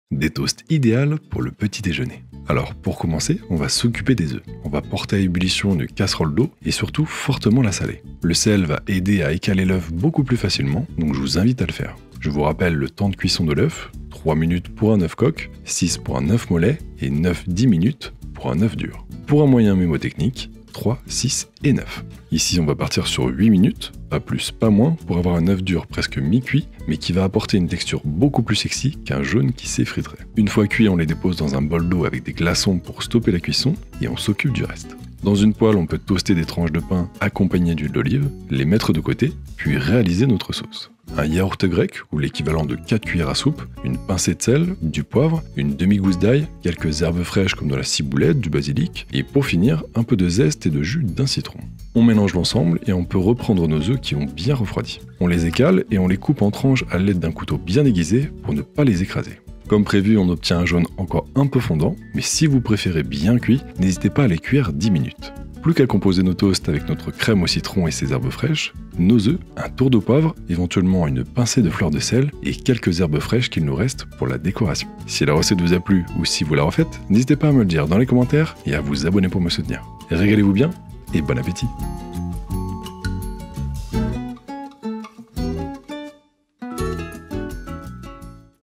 Un petit-déjeuner plein de fraîcheur et de saveurs ! 🍞🍳🌿
Une délicieuse façon de commencer la journée avec ces Toasts, une explosion de saveurs dès le matin ! 🍞🍳🌿

Ingrédients :
4 Cuillères à soupe de Yaourt grec
1 Cuillère à café d’huile d’olive
1 Demi gousse d’ail
Zeste et jus d’1 demi citron
Sel, poivre
2 Tranches de pain
2 Oeufs
Basilic et ciboulette